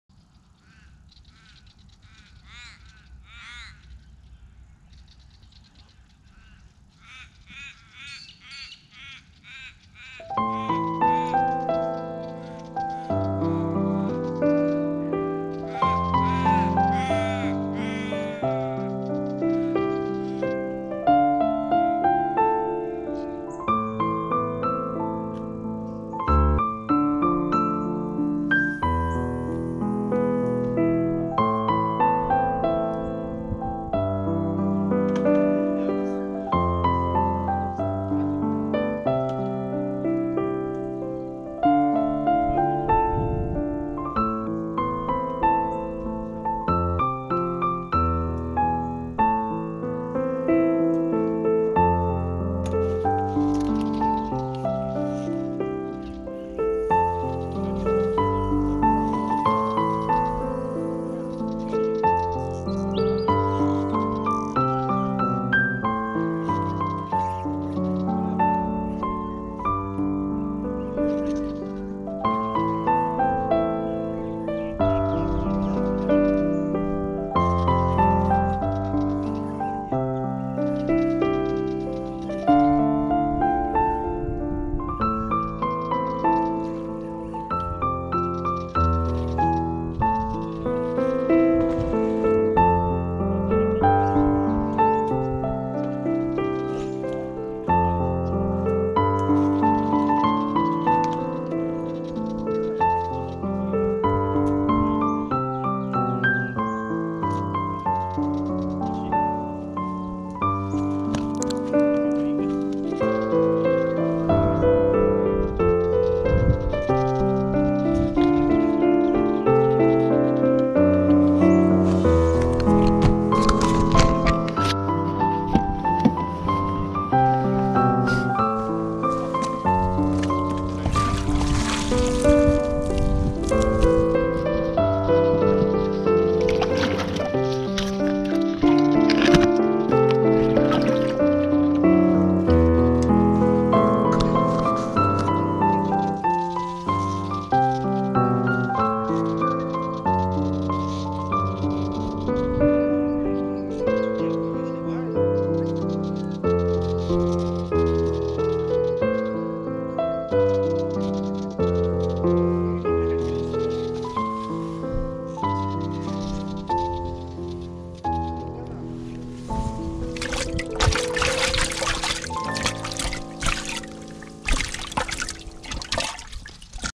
04 Dec 2020, Melbourne fishing. Bream
Fishing in a calm lake, different experience, but quite fun, as accidentally hit by a decent bream...